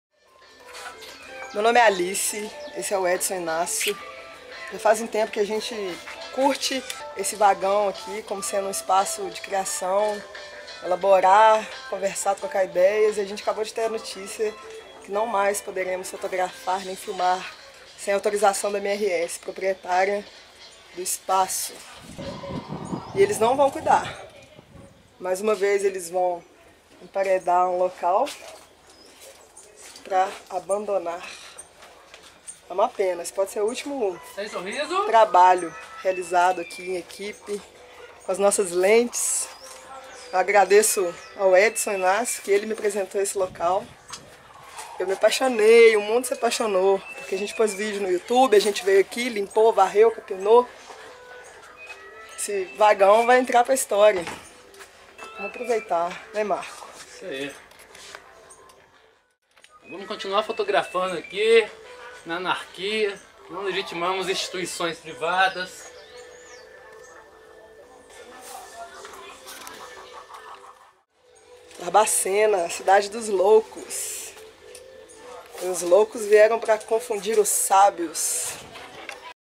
VAGÃO ABANDONADO OU NÃO ?
Depois de um tempo em que entrada e saída eram livres, fica a nota para os próximos visitantes/artistas/produtores que queiram realizar alguma atividade no vagão da MRS, que se localiza aos fundos da Estação Ferroviária de Barbacena ( que muitas vezes parece estar também abandonada a nossa Estação) :
A empresa agora envia seguranças ( assistencia autorizada") para impedir/inviabilizar os trabalhos, etc... e somente com a autorização expressa poder-se-á trabalhar em paz naquele local, tantas vezes visitado, "ocupado", admirado; querido laboratório de imagens... todas fantásticas!
A beleza está mesmo nos olhos de quem vê...